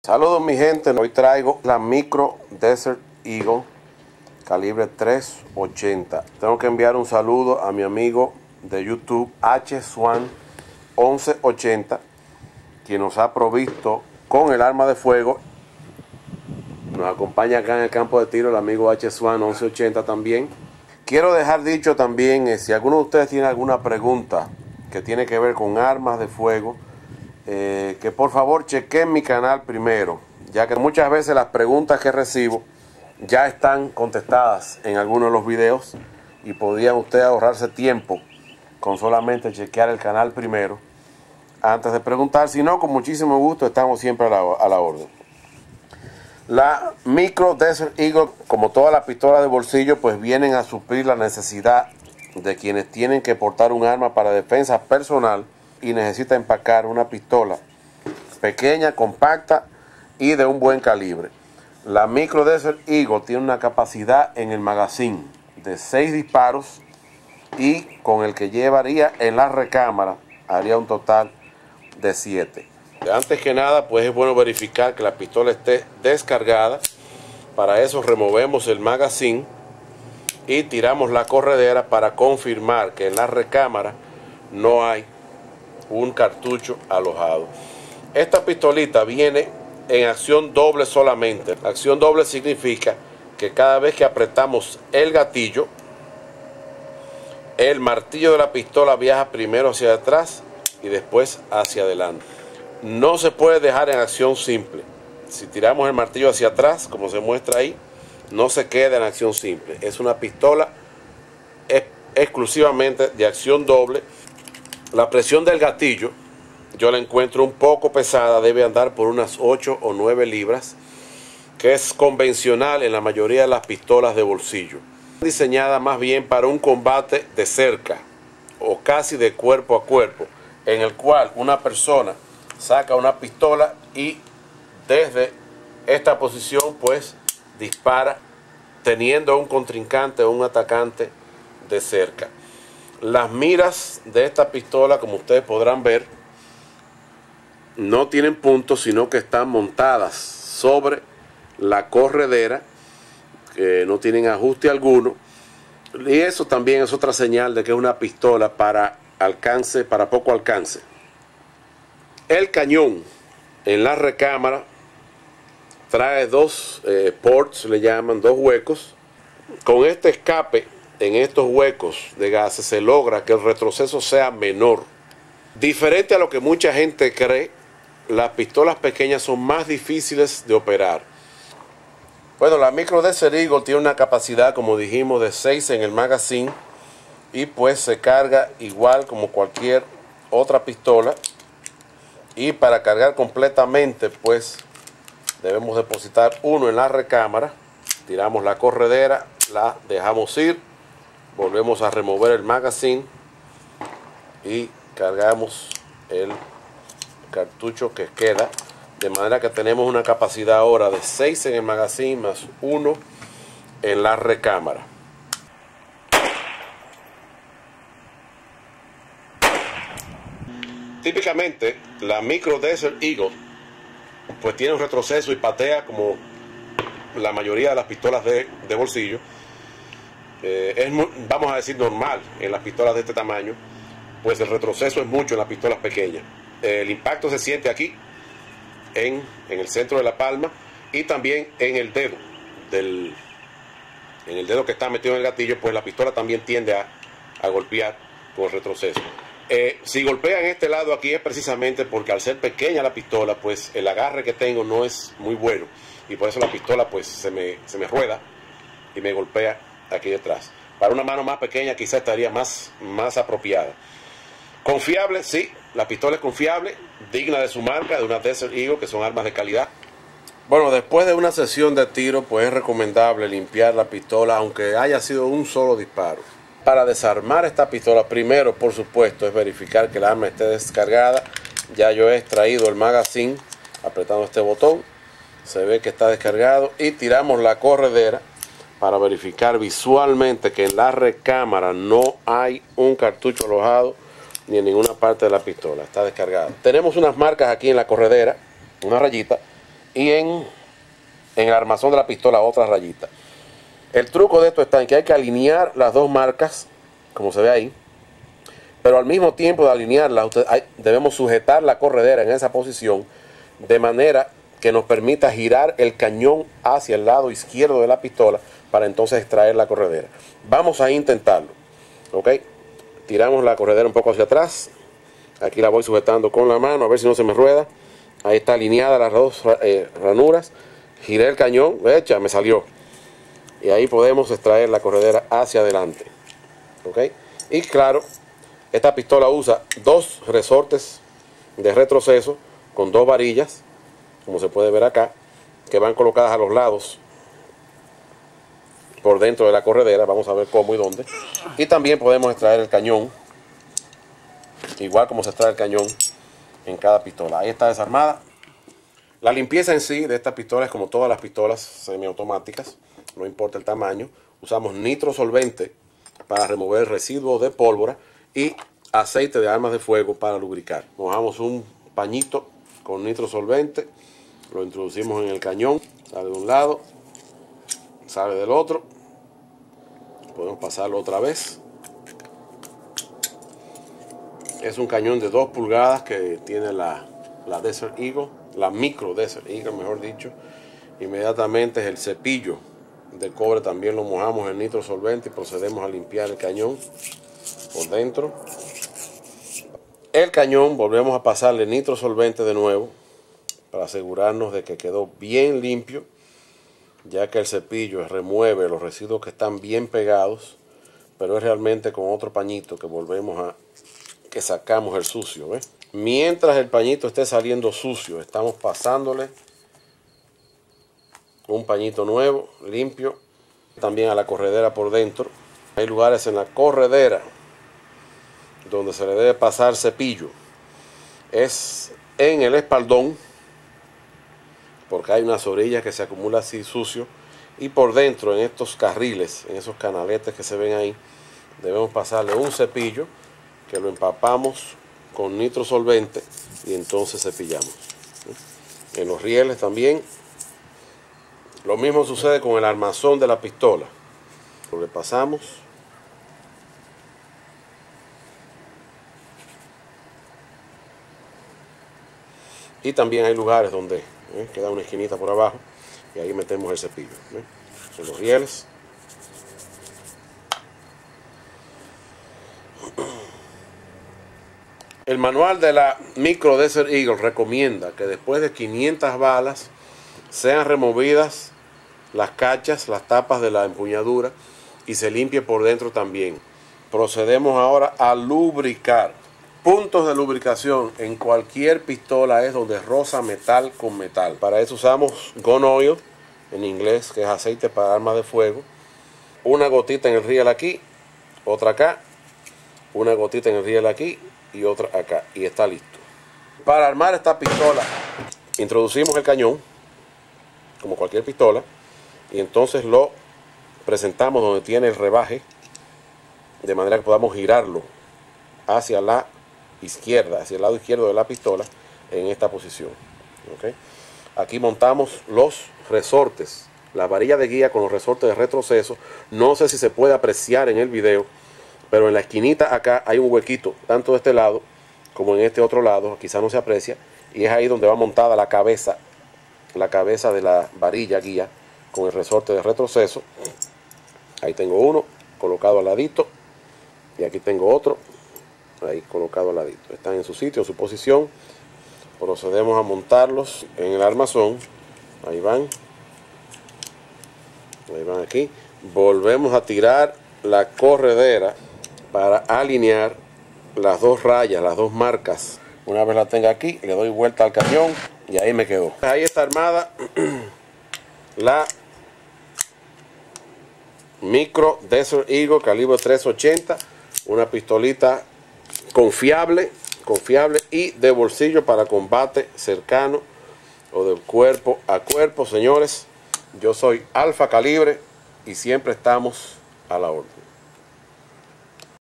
Micro Desert Eagle Calibre .380 - Pistola Sub-Compacta de Bolsillo
Pequeña, confiable, robusta. Esta pistolita de bolsillo tiene todos los buenos y malos modales de las pistolas de su clase en calibre .380.

Mi agradecimiento al amigo hswan1180 quien puso su pistola a la disposición para la realización de este video.